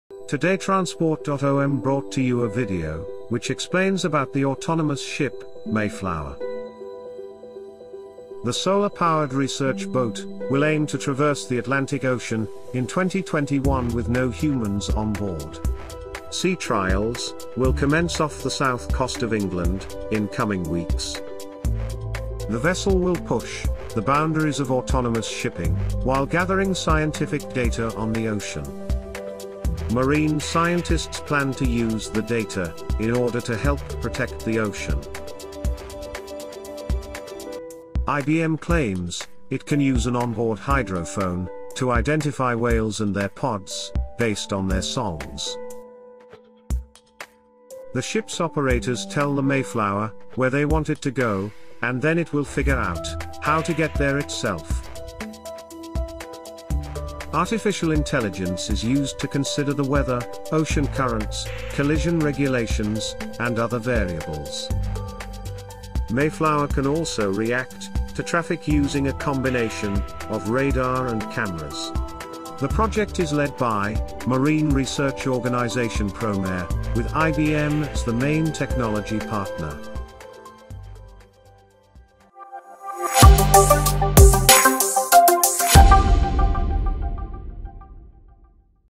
Autonomous ship 'Mayflower' -A Brief Description
The Mayflower Autonomous Ship (MAS) is a highly innovative project to design, build and sail the world's first full-sized, fully autonomous unmanned ship across the Atlantic Ocean.
The sailing of this ground-breaking new ship in 2021 will reflect the voyage undertaken 400 years ago when the original Mayflower left Plymouth to journey to America.
At over 100ft in length the Mayflower Autonomous Research will be powered by state-of-the-art wind and solar technology. The revolutionary trimaran vessel will carry on board a variety of drones through which it will conduct experiments during its voyage.
It was officially launched at a naming ceremony in Plymouth on 16 September 2020, 400 years after the original Mayflower left the maritime city to cross the Atlantic.